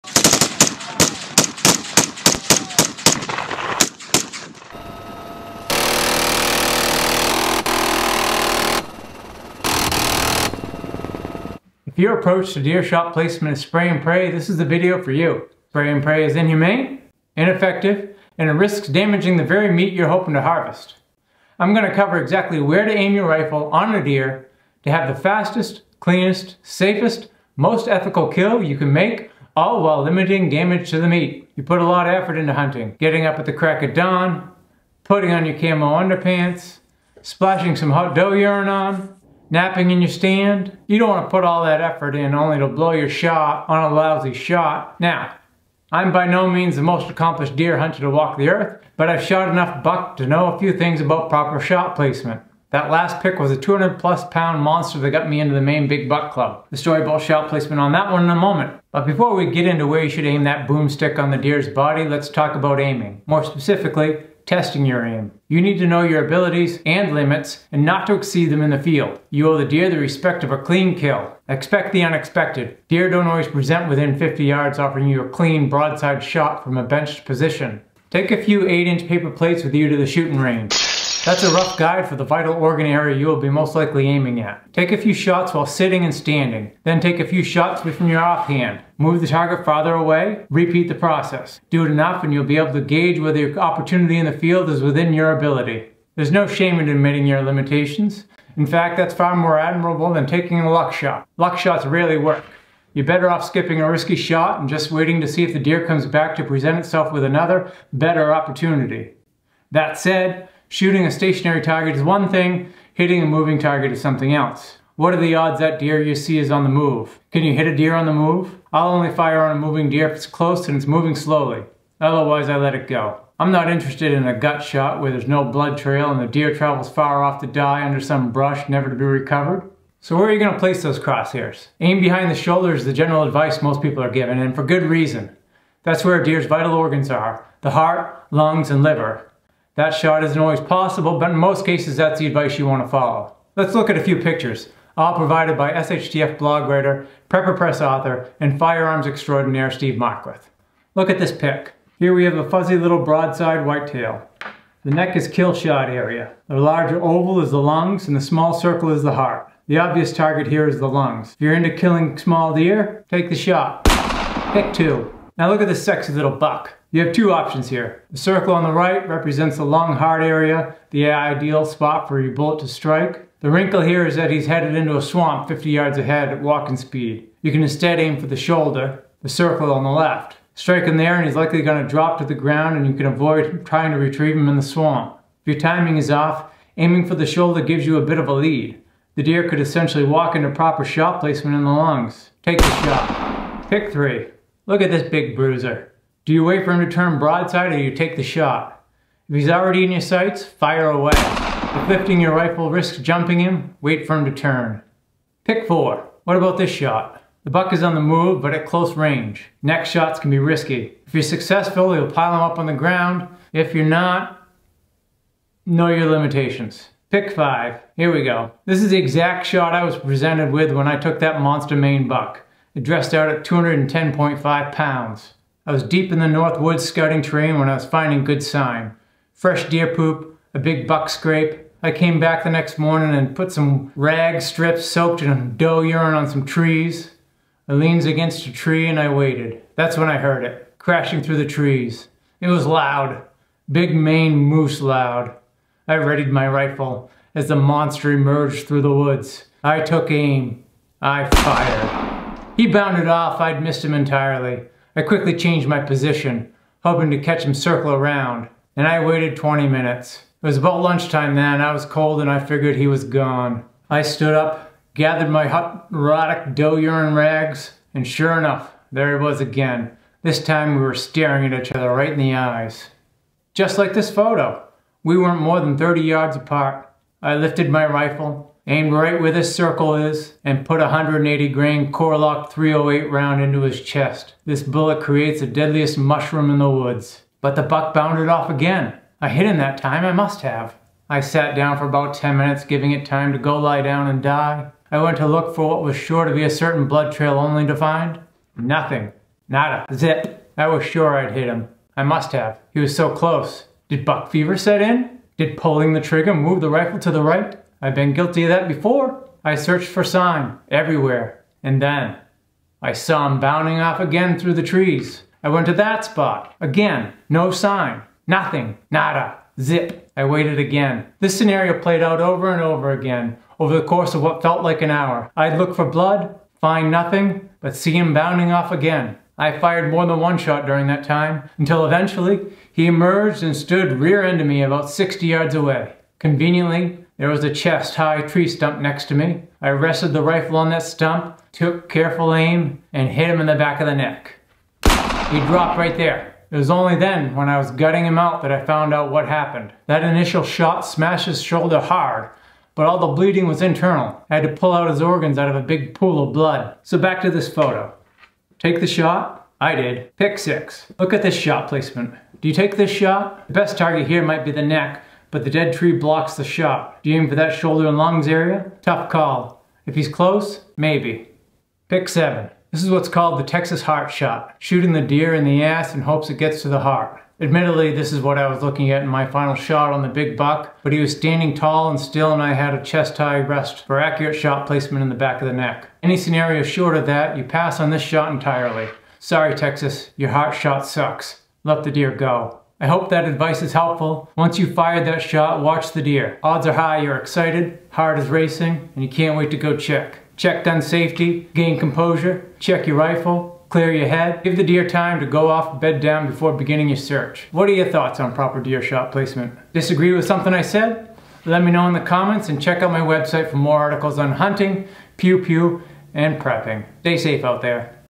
Proper Deer Shot Placement - 7 Examples
Deer shot placement is a question newer hunters are taught by fathers, grandfathers, and even hunter safety courses. “Aim behind the shoulder” works assuming a convenient broadside opportunity, but that’s not a guarantee. Here I cover where to properly aim when hunting deer.